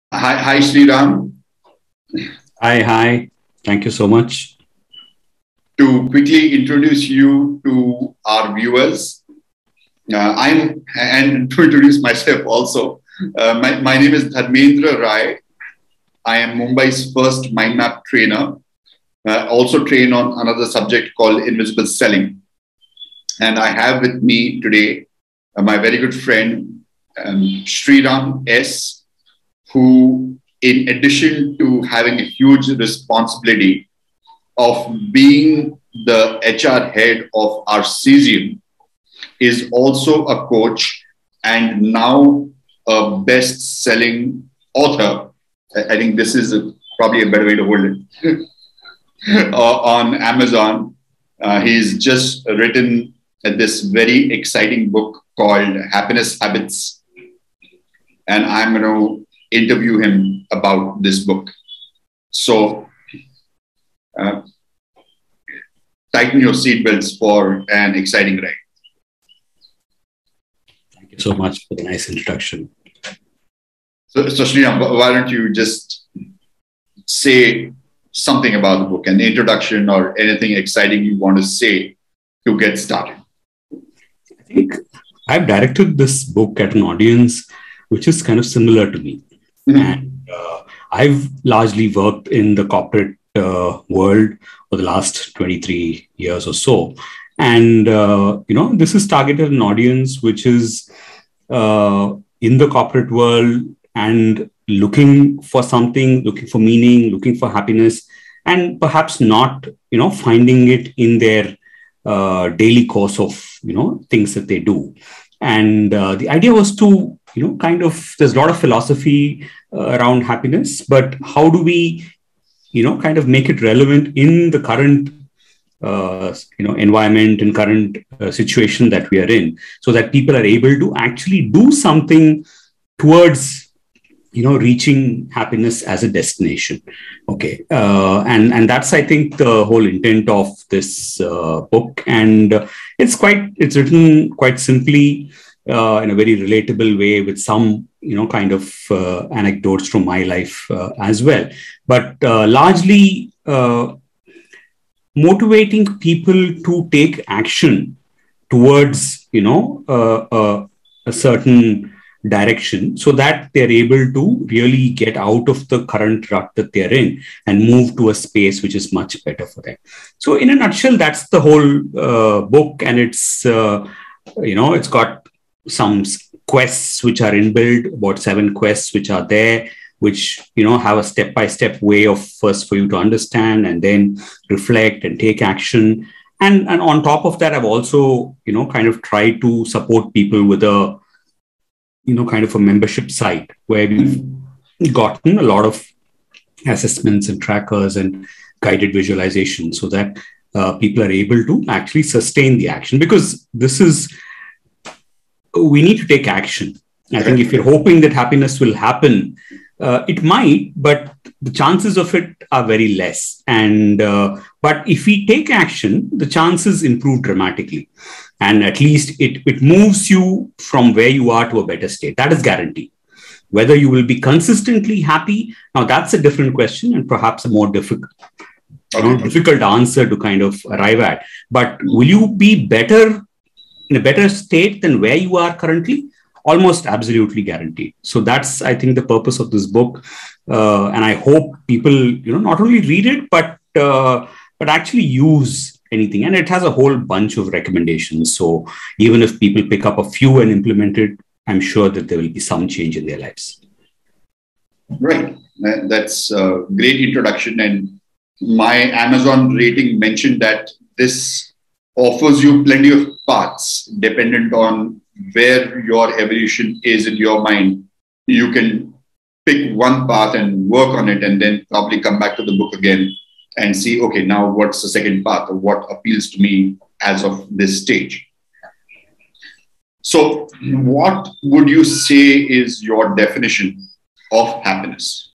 D Rai Interviews Sriram S Happiness Habits 1
Dharmendra Rai, Mumbai 's First Mind Map Trainer , Interviews Sriram S , Director & Head HR , Arcesium | Author- Happiness Habits | Speaker | Coach on his book Happiness Habits Part 1 - INTRODUCTION